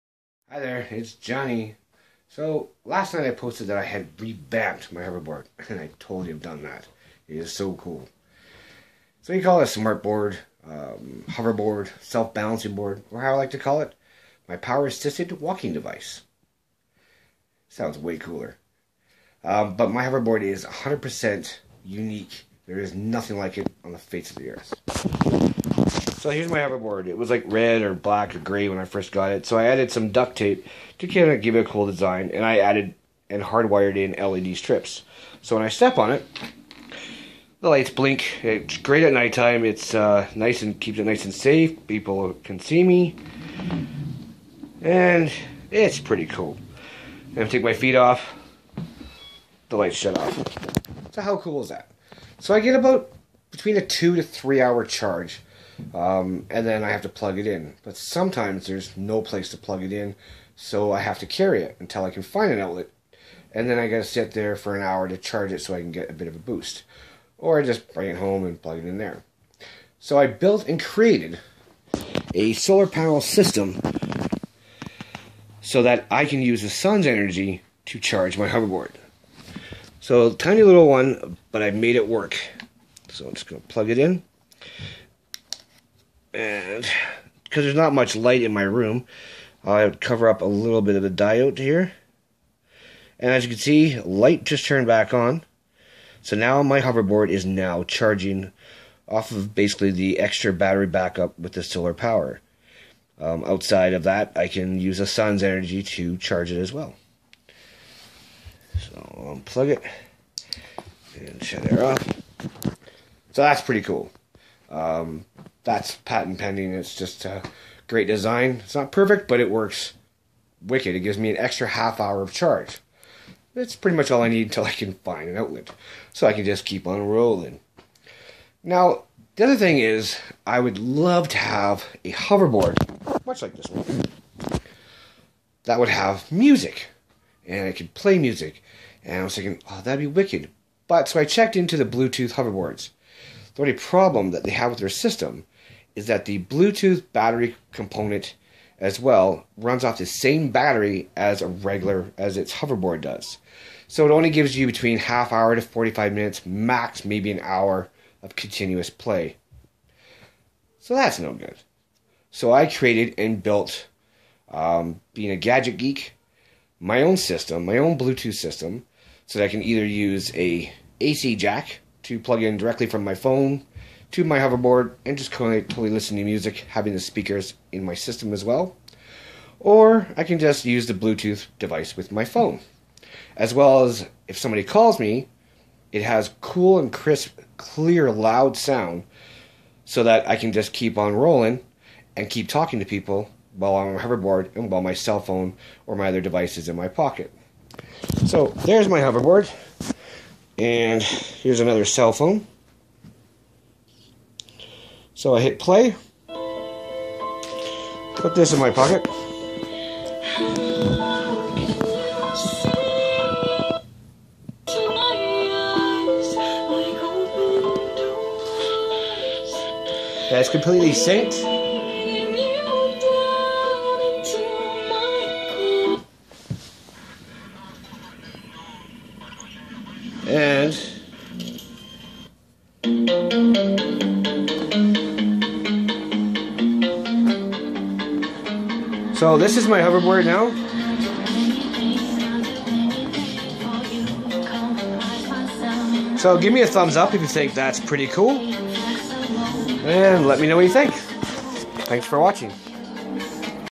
Hoverboard charger and Bluetooth
Built and created a solar charger and Bluetooth for my hoverboard